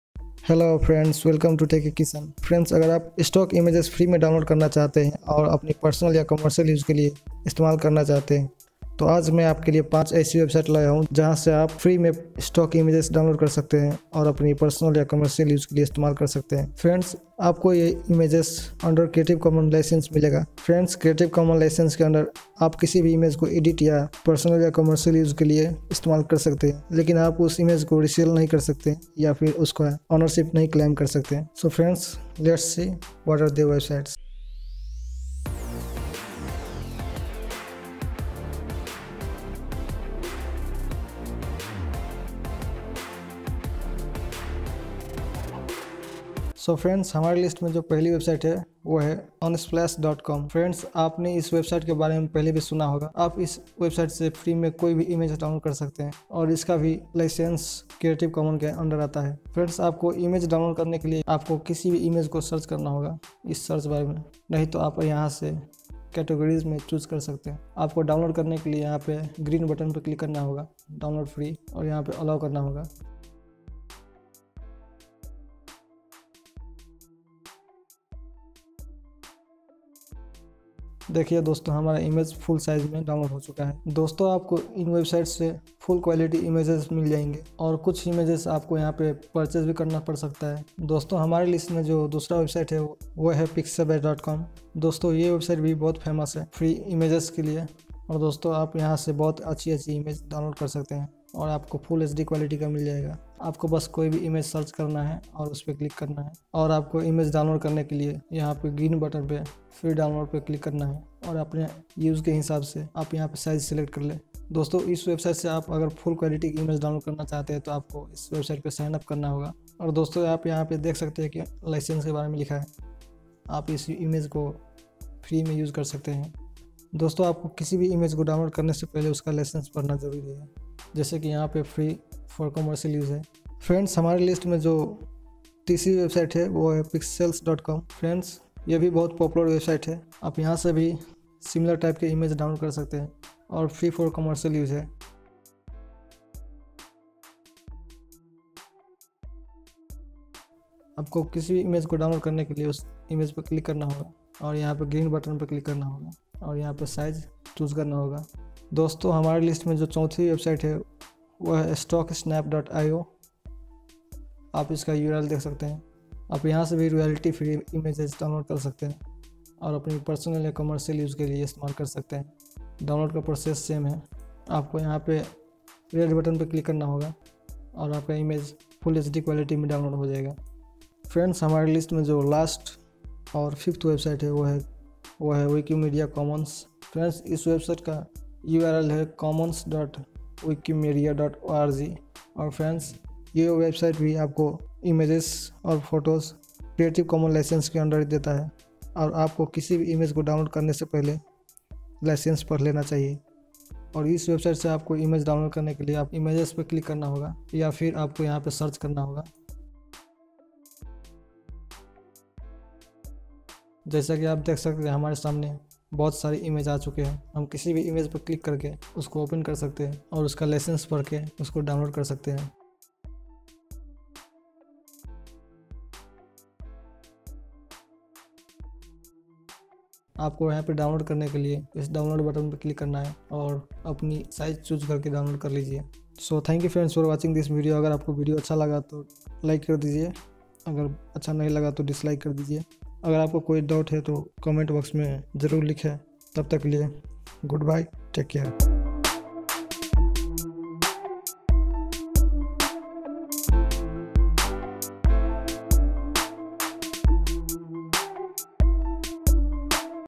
Royalty Free Stock Images For Commercial Use | 5 websites
Here are 5 websites to download  royalty free images for personal and commercial uses.